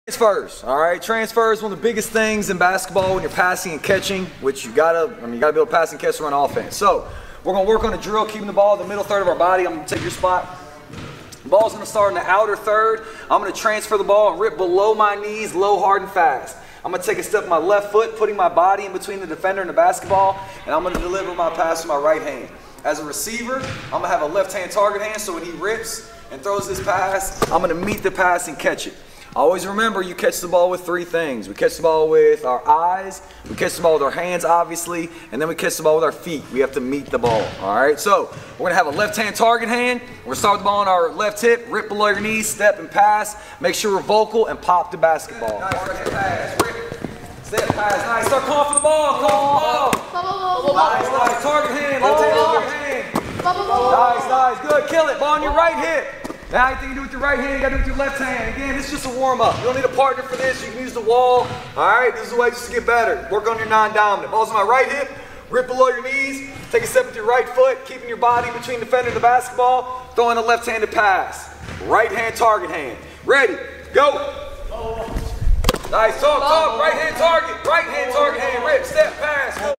“Transfer Passing”
You must be able to pass and catch to run offense‼️Simple drill to work on many micro skills (ball out of mid 1/3, violent rip, angle of step, target hand, etc…) that are great for development🏀 “Transfer Passing”